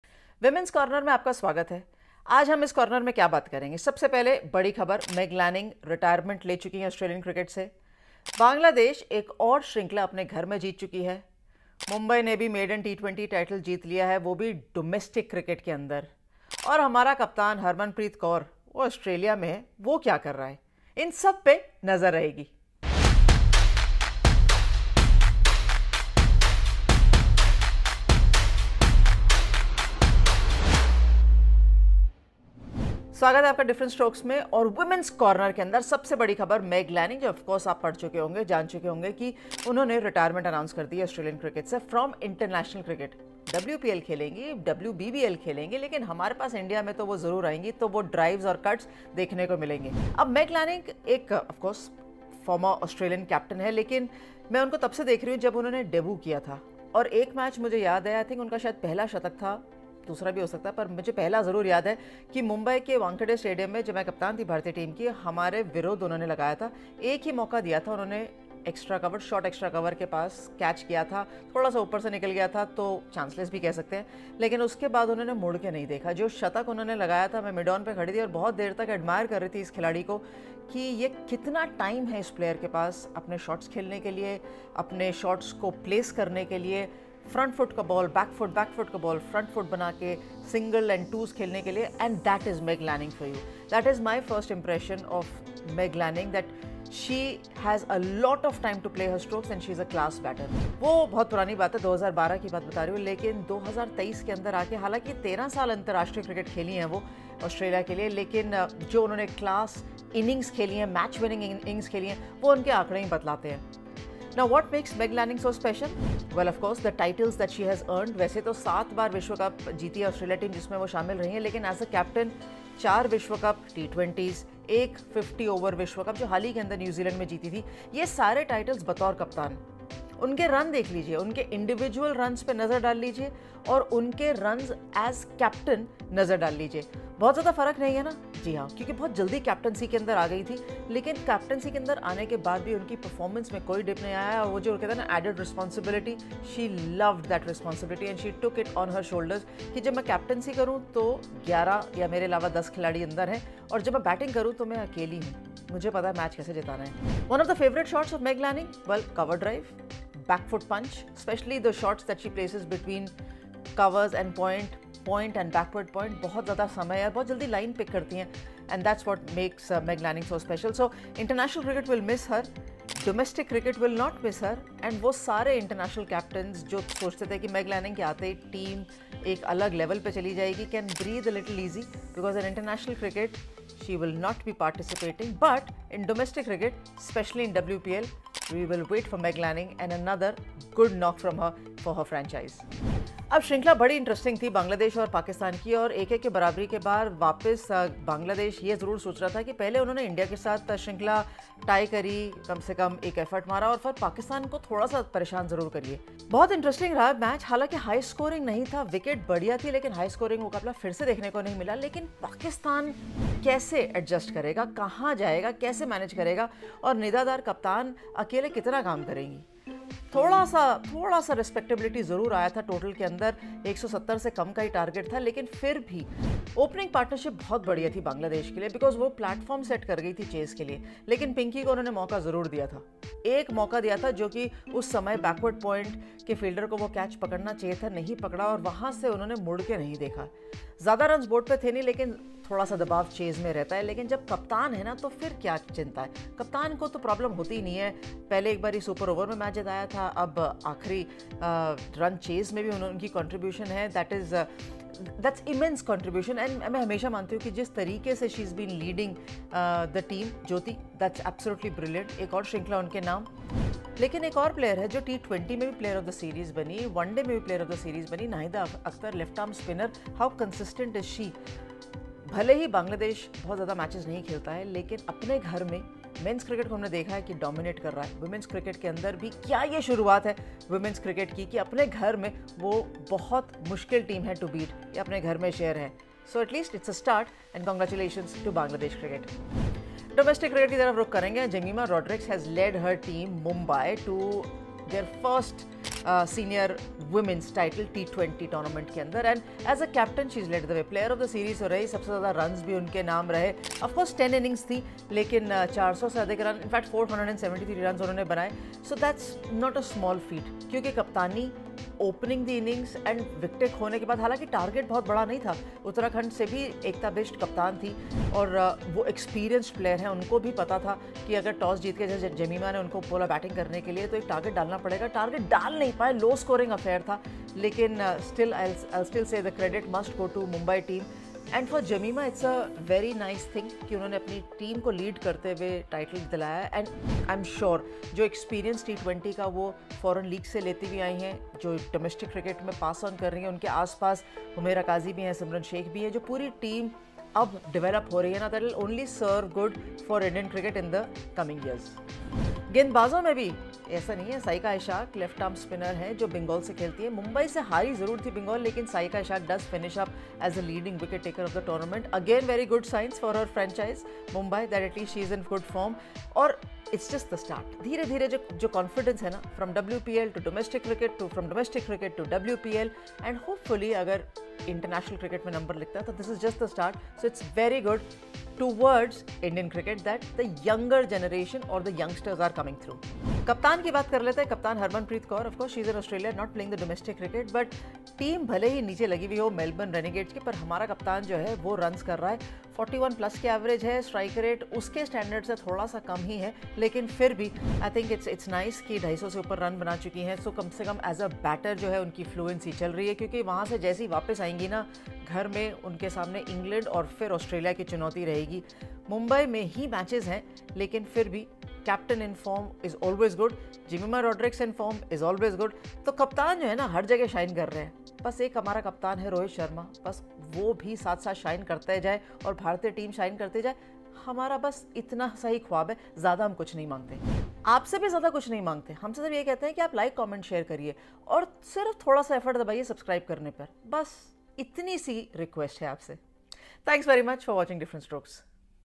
Meg Lanning's retirement from international Cricket | Women's Cricket | Anjum Chopra
A stellar of a career from Meg Lanning comes to an end as she announces her retirement from International cricket! Meanwhile Jemimah Rodrigues leads Mumbai to their maiden victory in the Senior Women's T20 League. All that in today’s episode of differentstrokes